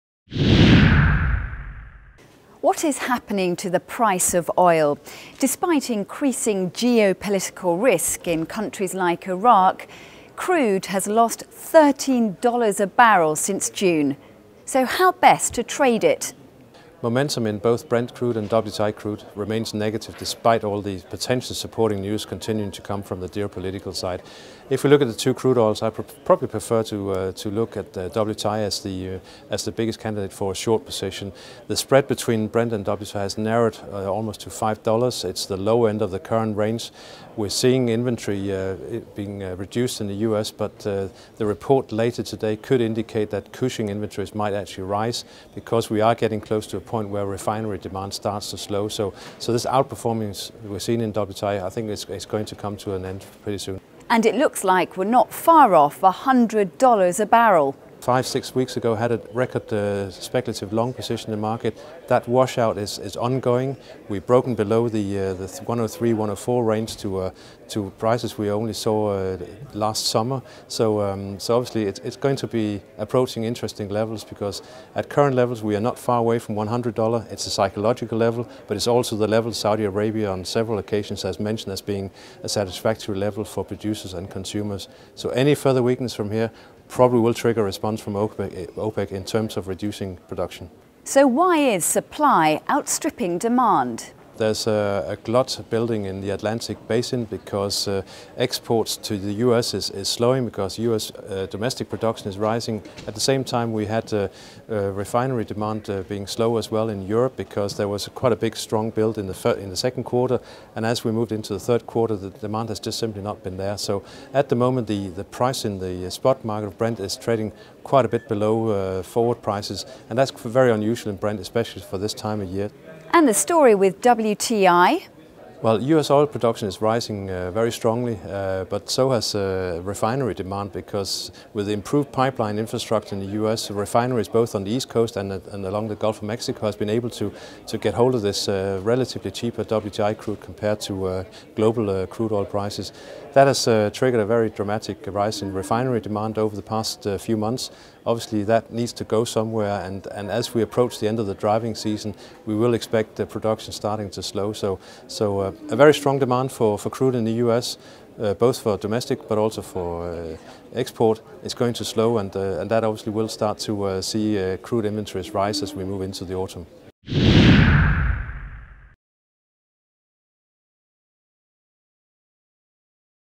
As the oil price falls how best to trade crude
U.S. oil prices posted their largest drop in almost two years and the global Brent contract fell into a bear market on Tuesday after OPEC oil supplies reportedly exceeded demand.

Oil prices were also under pressure as traders squared up books with the last trading day of the month and the quarter, and on the expiration of contracts for refined fuels.